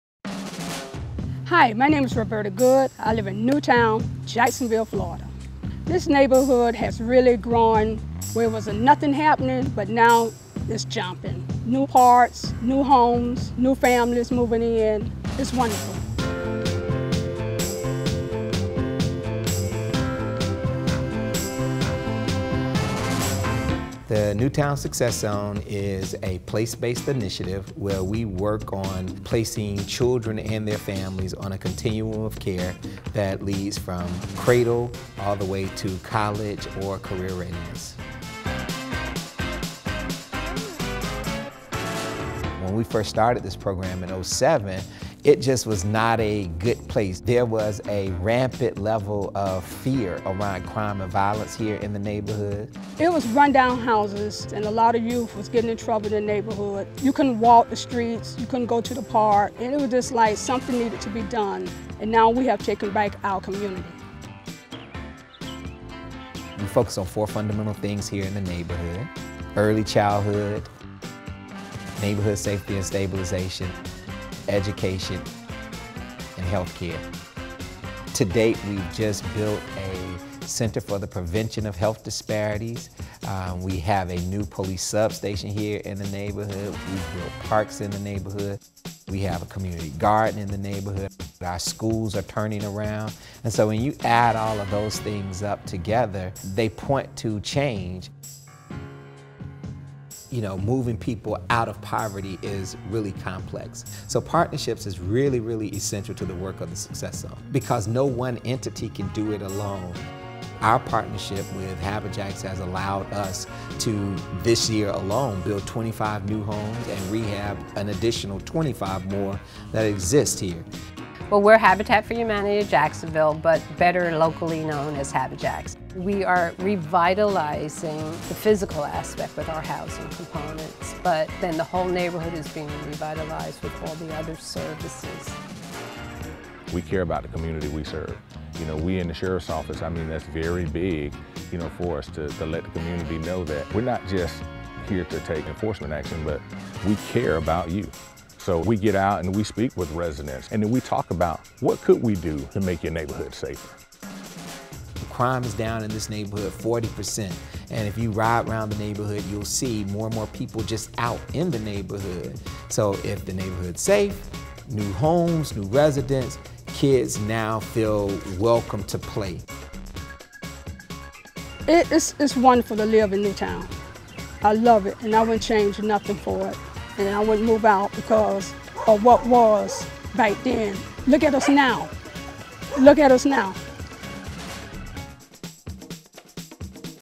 Habitat for Humanity's Neighborhood Revitalization helps improve Jacksonville's New Town community
Look at the positive outcome in the New Town neighborhood of Jacksonville, Florida, where safety, health, education and housing have been improved through partnerships with homeowners and other community organizations. Read more about the New Town Success Zone partnership